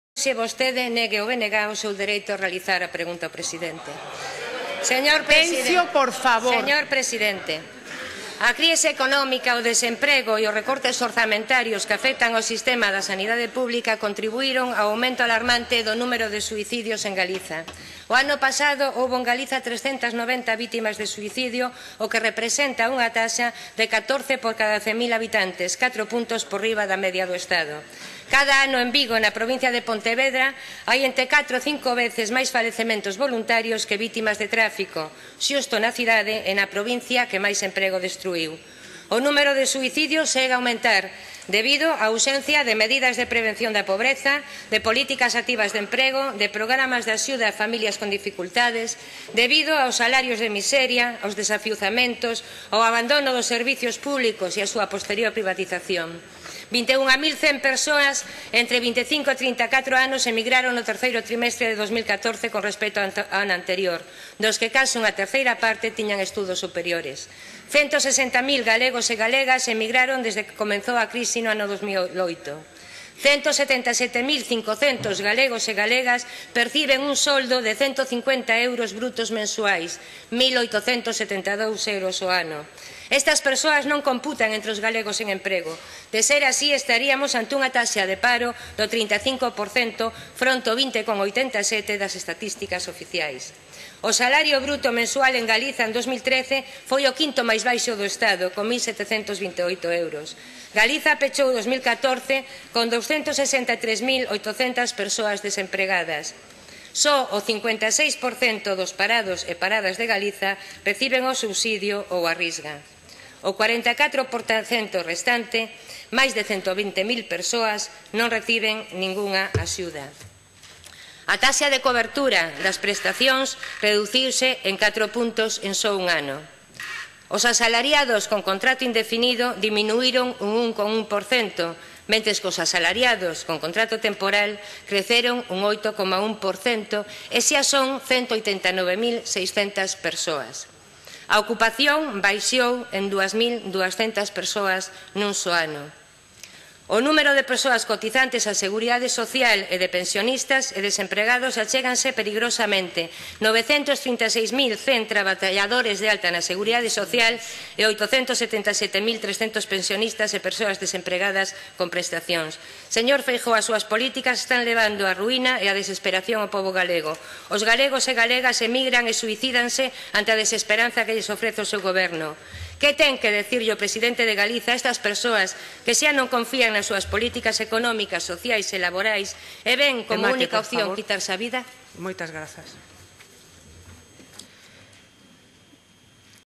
"Que ten que dicir Feijóo ante a alta taxa de suicidios en Galiza?"
Chelo Martínez pregúntalle a Feijóo qué ten que dicir ante a alta taxa de suicidios en Galicia, moitos provocados polas súas políticas.

Os recortes e a crise provocaron un aumento dos suicidios en Galiza, sumida nunha grave crise social na que 160.000 galegos e galegas emigraron dende que comezou a crise en 2008 e cunha taxa real de paro do 35% se contamos ás persoas con salario insuficiente para sosterse.

Chelo Martínez solidarízase co BNG e lamenta que o PP lle negue o seu dereito a formular a pregunta ao Presidente.